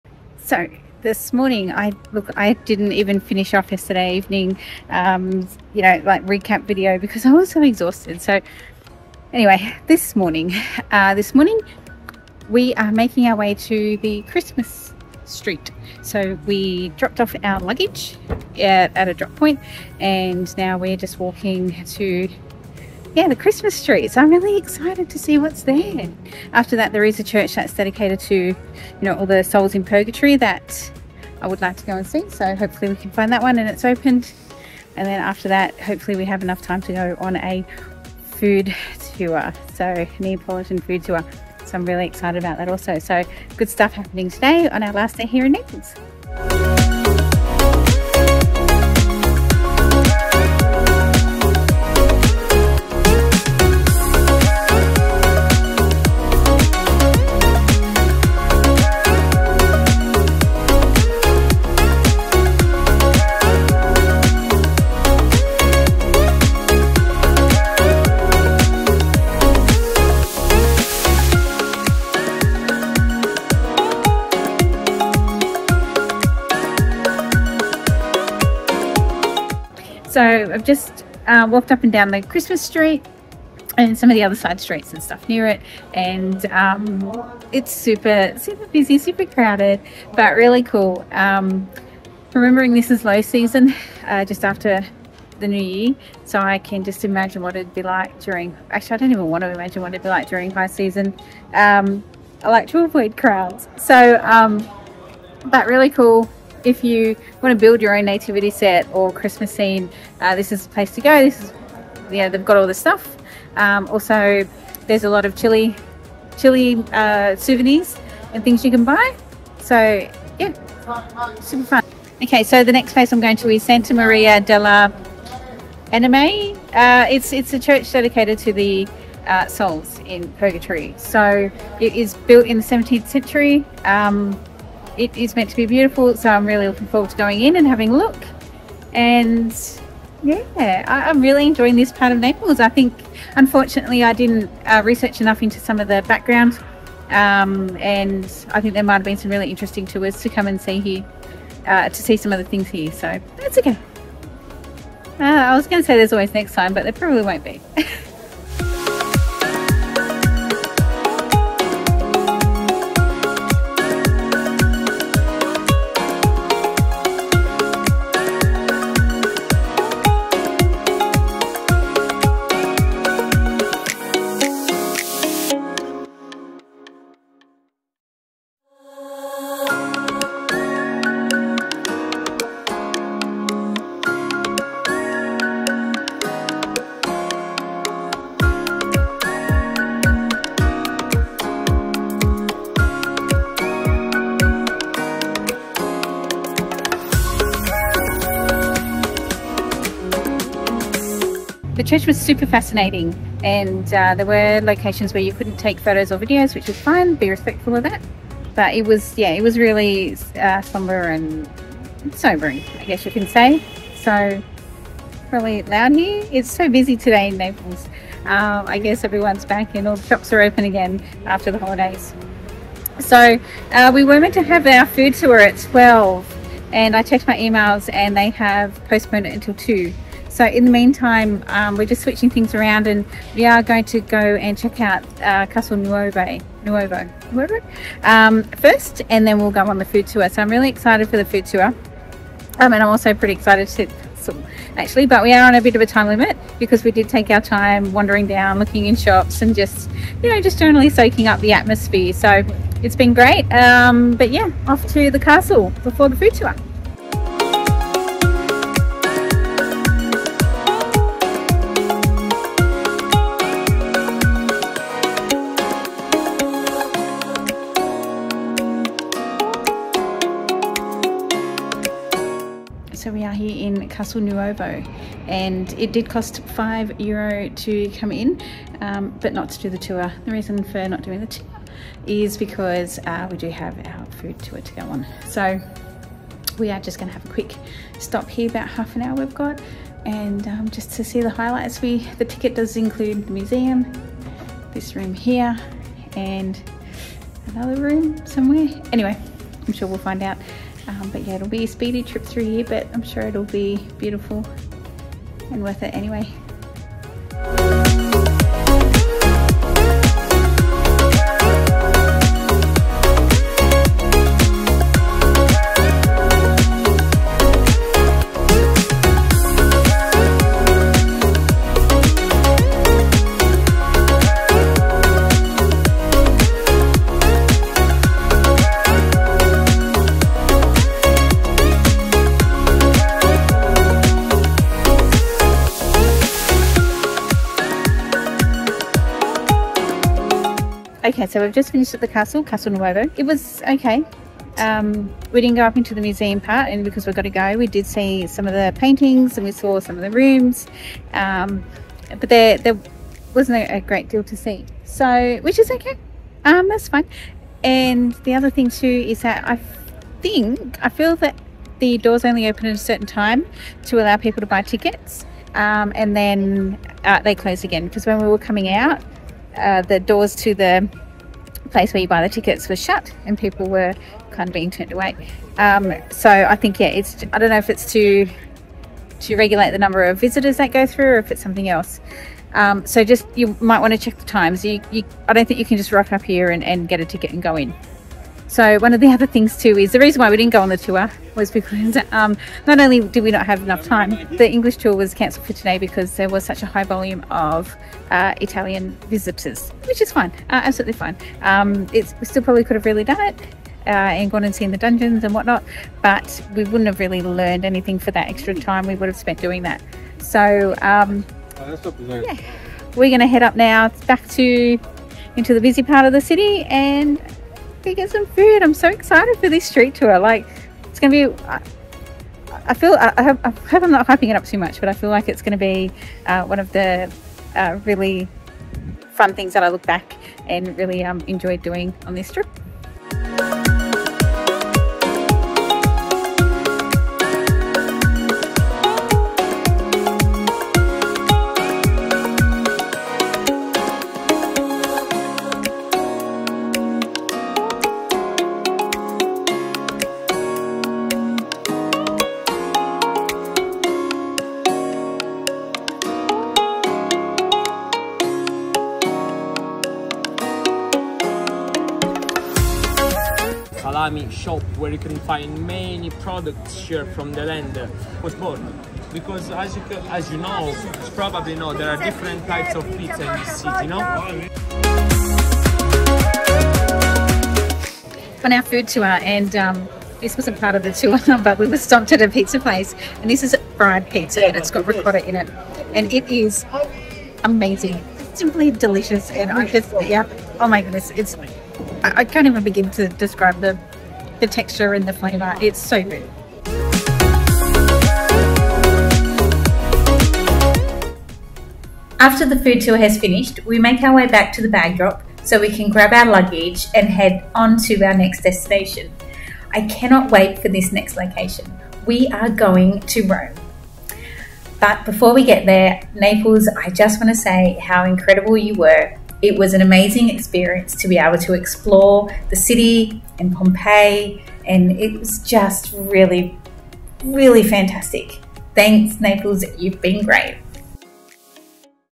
Christmas Street and a delicious Naples street food tour - Exploring Italy Part 4
Naples is such a city of vibrant culture, history, and gastronomy. It's been a wonderful experience to be able to explore this amazing part of Italy.

So, join me as I step into a world where history, mystery, and flavour collide. Buckle up, fellow explorers, because this is no ordinary escapade—it’s an unforgettable odyssey through the heart of this vibrant Italian gem.

Christmas Alley: A Festive Wonderland
Take a stroll down San Gregorio Armeno, affectionately known as Christmas Street. Here, artisans weave magic with their hands, sculpting intricate Nativity scenes that transport us back in time. The narrow cobblestone lanes come alive with miniature shepherds, wise men, and angels—all meticulously crafted. It’s a Nativity tradition that has spanned centuries.

Santa Maria delle Anime del Purgatorio ad Arco: Where Souls Await Redemption
Our next stop takes us to the Complesso Museale Santa Maria delle Anime del Purgatorio ad Arco—a name that rolls off the tongue like a haunting melody. Here, lost souls seek solace, their earthly burdens lightened by prayers. Standing in front of the altar, one ponders mortality and the afterlife. It’s eerie yet strangely comforting—a place where faith and art intertwine.

Castel Nuovo: Where Legends Echo
Behold the majestic Castel Nuovo, rising like a sentinel against the azure sky. Its medieval stones guard tales of kings, battles, and dynasties. There is an amazing view of the Gulf of Naples. Unfortunately, the castle has more to offer than I had time for.

Street Food Extravaganza: A Symphony of Flavours
Our senses lead us to the bustling streets, where aromas dance and flavours collide. Naples’ street food is an orchestra of taste, and we’re front-row spectators. Experience bread, fried foods, salami, olives, pizza, limoncello and coffee! This is Naples at its most authentic.

From Nativity scenes to ancient keeps, from hidden chapels to savoury bites, this city defies expectations. So, fellow wanderer, immerse yourself. Naples awaits, ready to steal your heart, one cobblestone at a time.

00:00 Introduction
00:46 Christmas Alley
02:39 Complesso Museale Santa Maria delle Anime del Purgatorio ad Arco
04:17 Castel Nuovo
07:13 Naples Street Food Tour